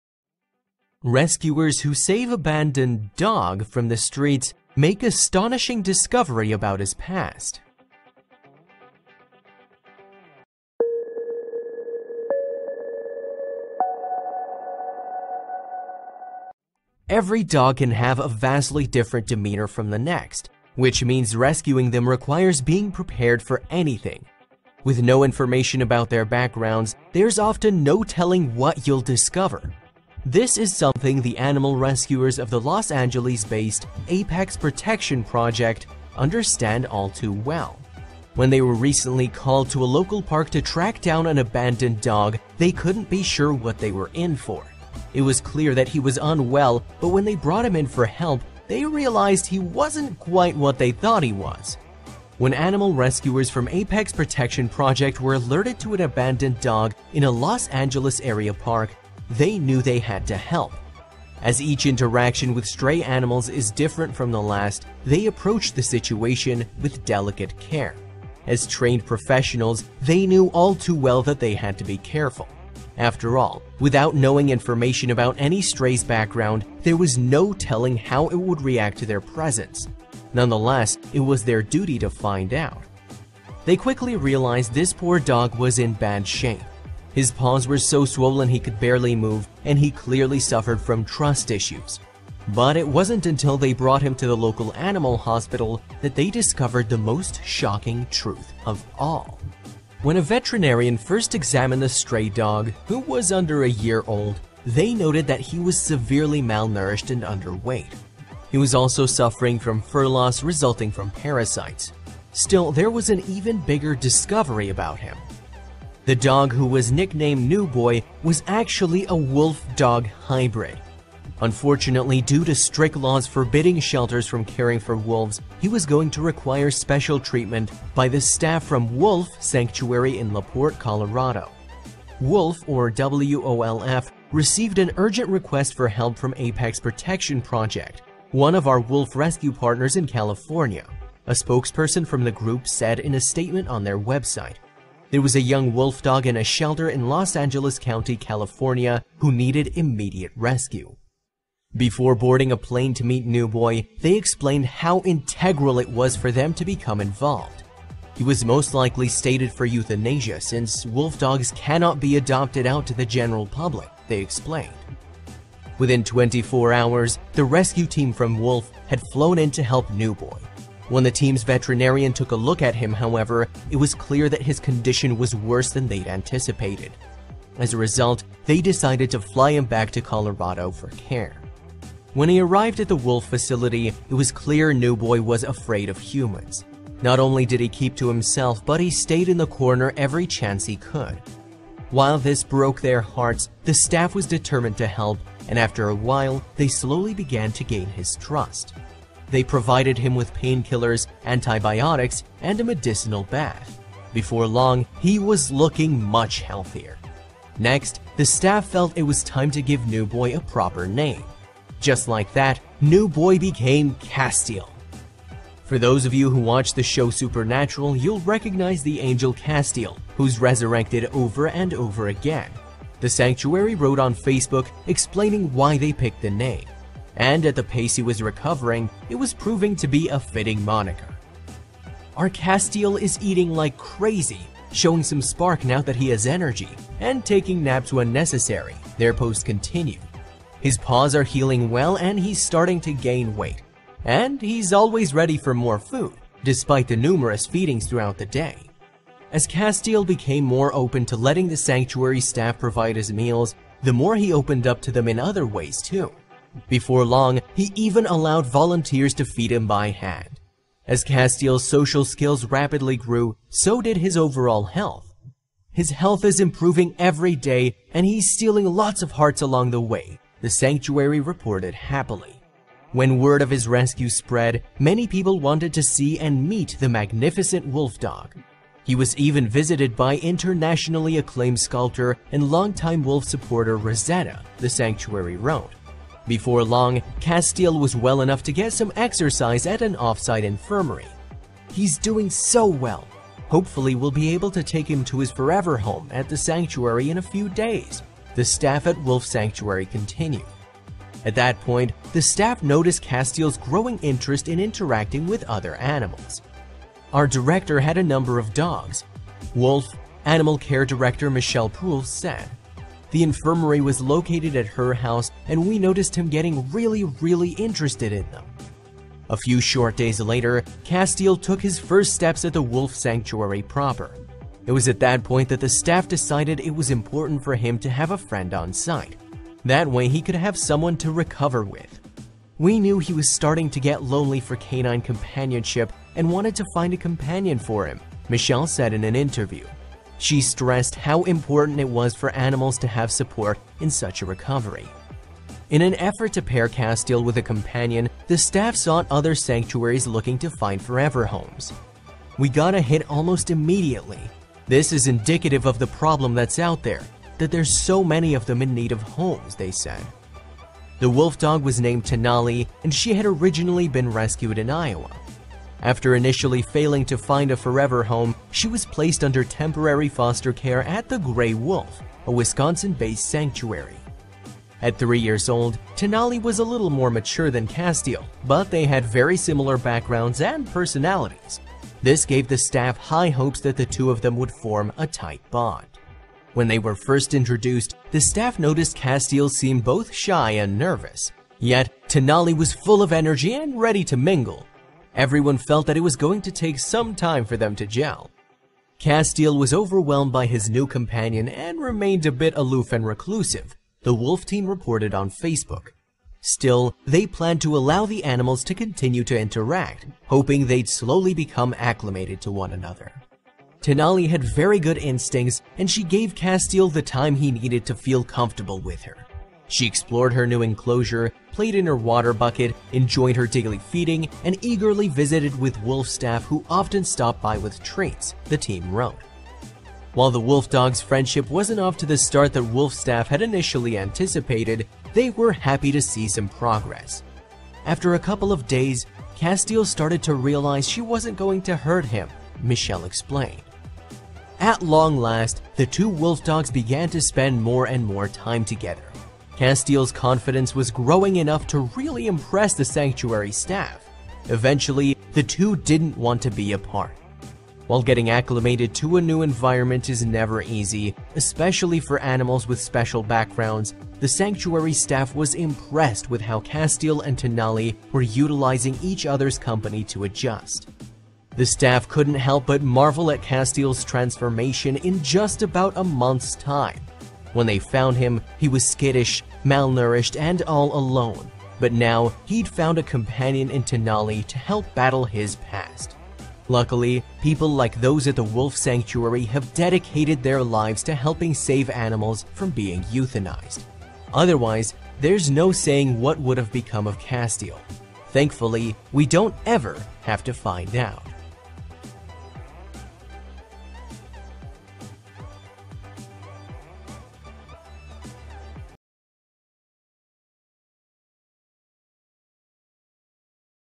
Rescuers Who Save Abondoned 'Dog' From The Streets Make Astonishing Discovery About His Past
Every dog can have a vastly different demeanor from the next, which means rescuing them requires being prepared for anything. With no information about their backgrounds, there’s often no telling what you’ll discover. This is something the animal rescuers of the Los Angeles-based Apex Protection Project understand all too well.

When they were recently called to a local park to track down an abandoned dog, they couldn’t be sure what they were in for. It was clear that he was unwell, but when they brought him in for help, they realized he wasn’t quite what they thought he was…